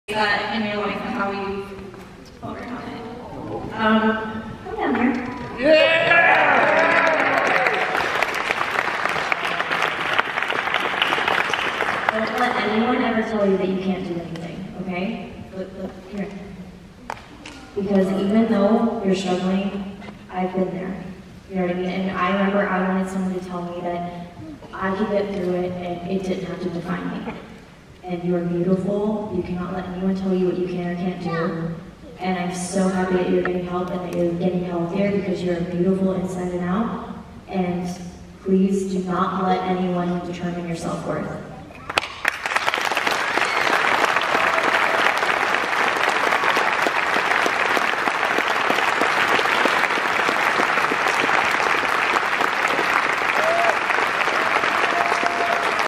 Alexa Bliss mentors young fan during WWE panel @ MegaCon Orlando 2018
MegaConOrlando2018 - Aqui el momento emotivo del panel de las superestrellas de la WWE en MegaCon Orlando 2018 donde Alexa Bliss aconseja, y termina abrazando a una joven fanatica luego de contestar una de sus preguntas. LittleMissBliss Humble 💪💪💪